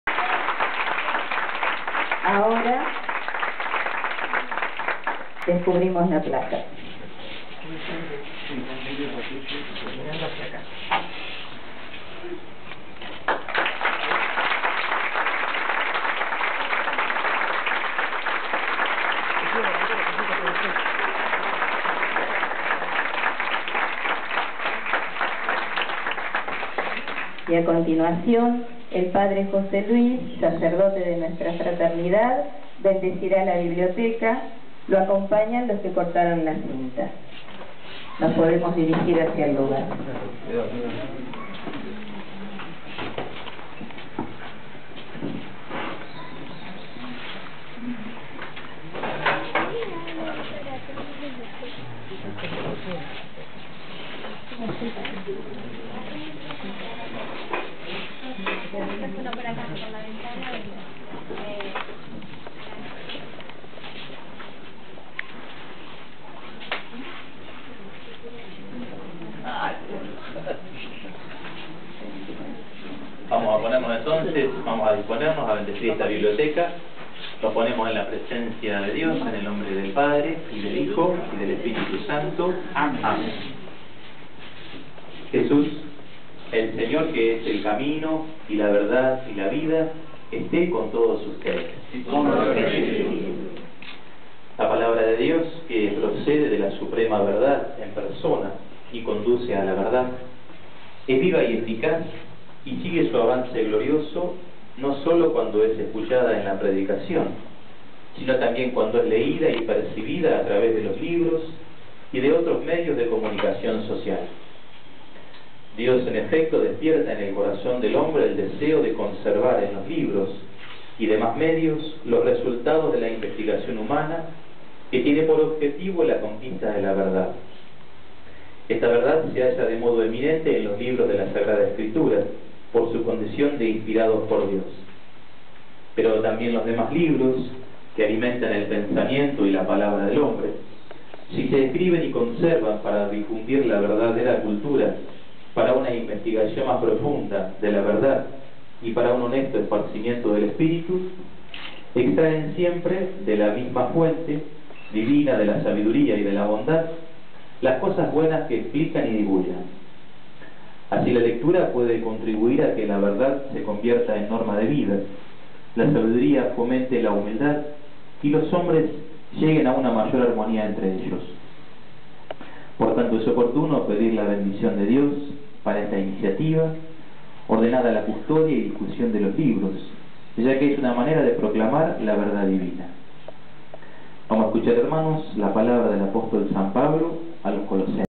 INAUGURACION BIBLIOTECA ANGELO CHIATTO
Acto en el Colegio Divino Maestro de san Gregorio